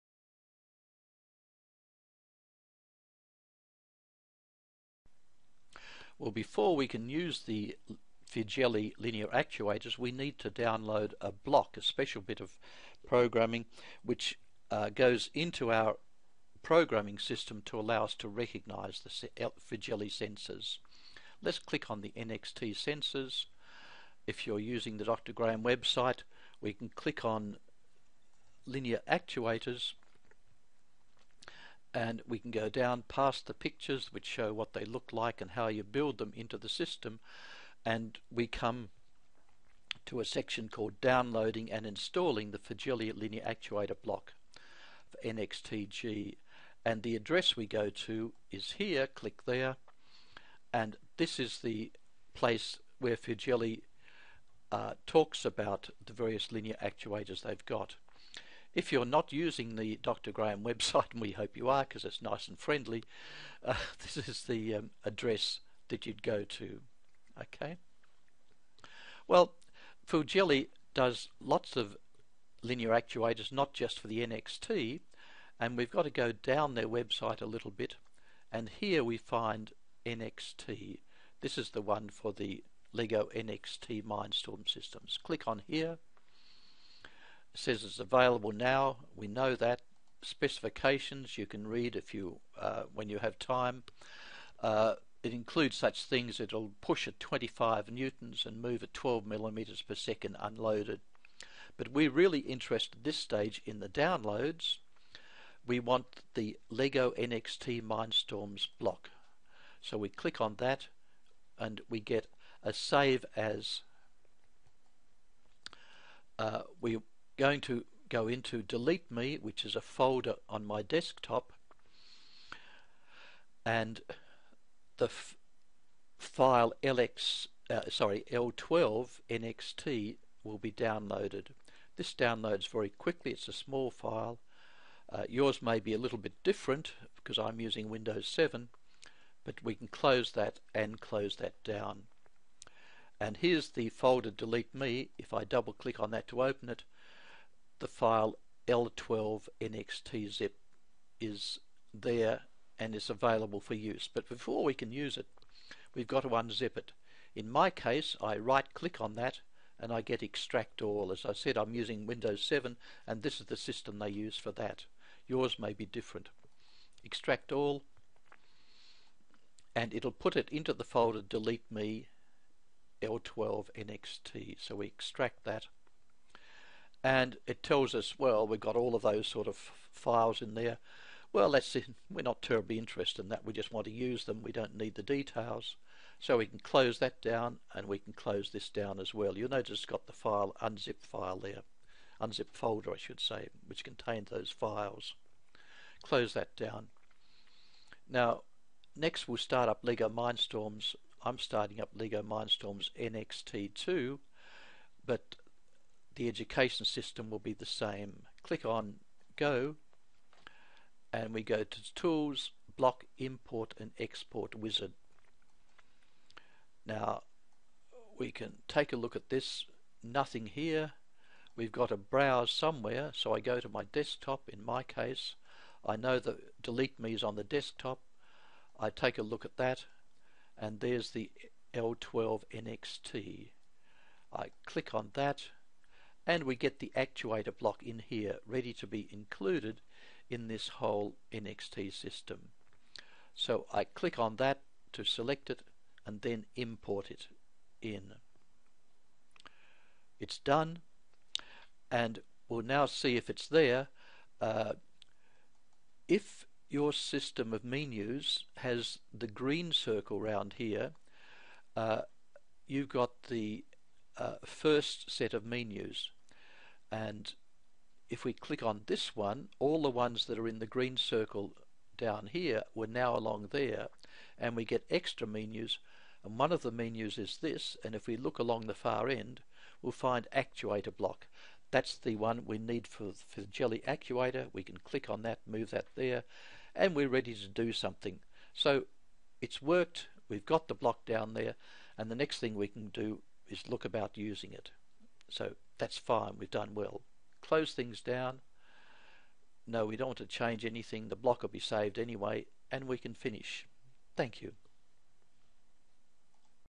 Getting the NXT-G block, Firgelli Linear Actuator with NXT
This video is the third of 5 in a series "Introducing the Firgelli Linear Actuator", in this case demonstrating the process of adding the NXT-G block necessary for the Firgelli linear actuators to be used with LEGO's NXT Mindstorms system.
The rest of the videos in this free series (plus some free introductory NXT-G tutorials) can be seen at DrGraeme.net/s.htm